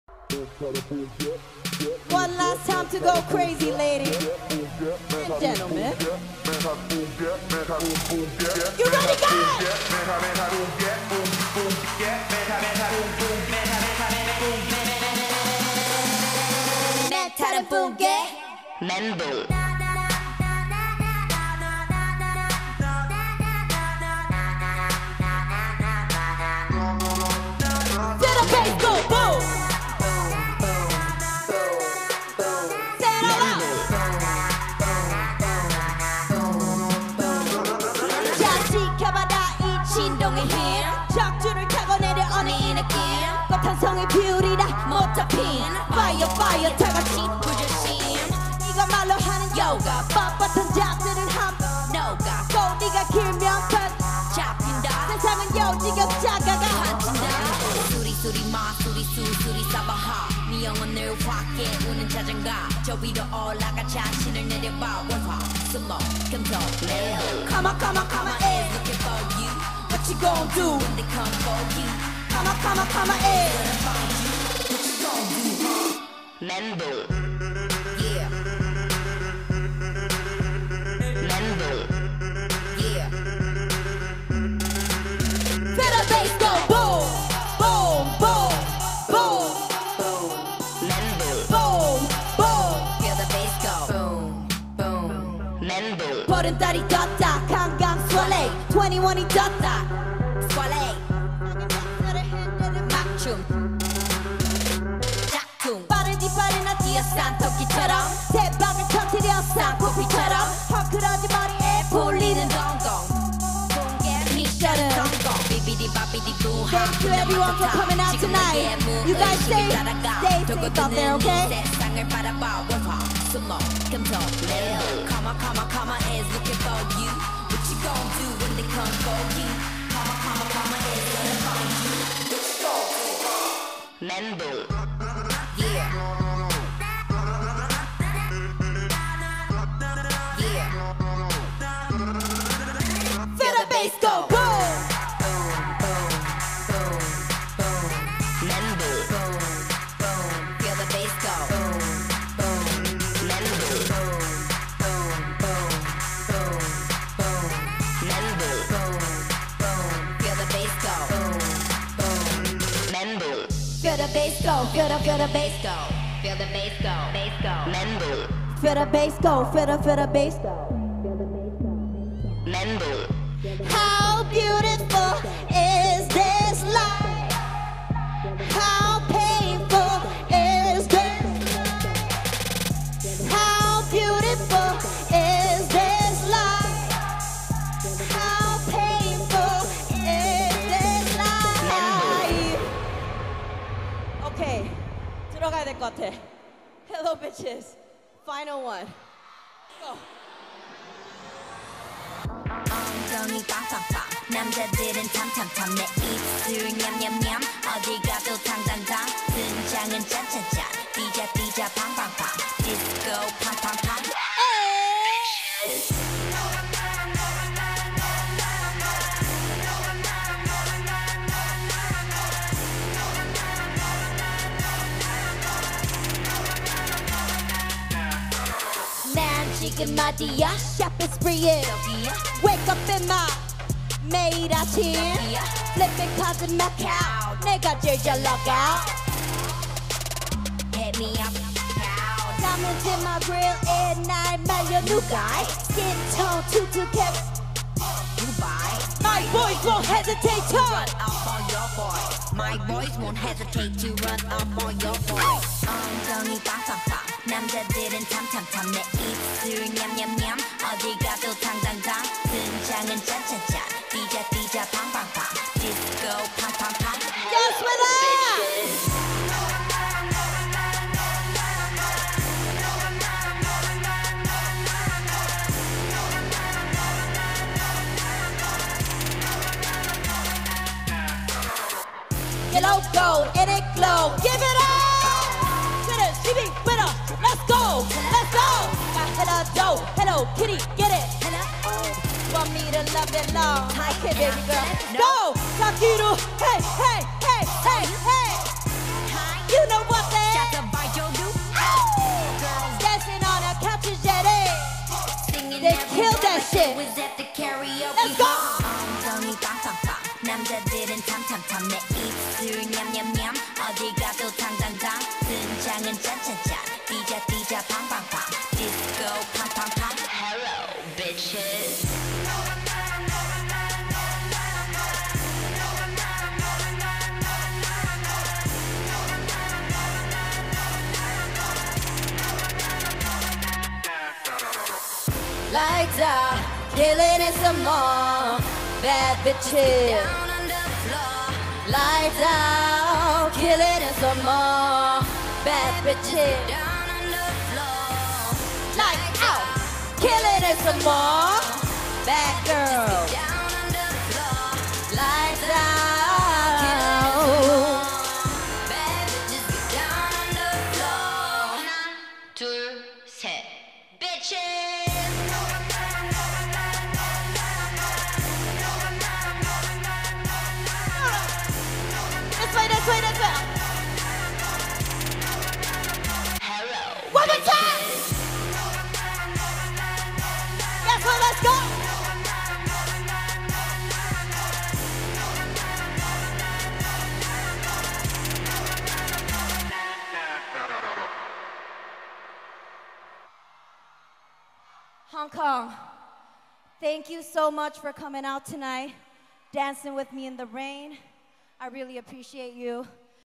CL《Star Nest Music Festival》
CL壓軸登台全場High爆，雖然雨勢開始頗大，但無礙觀眾高呼狂叫，CL亦好識帶動現場氣氛，叫觀眾舉手一齊Rap 、一齊唱，CL迅速與台下觀眾打成一片，十分興奮。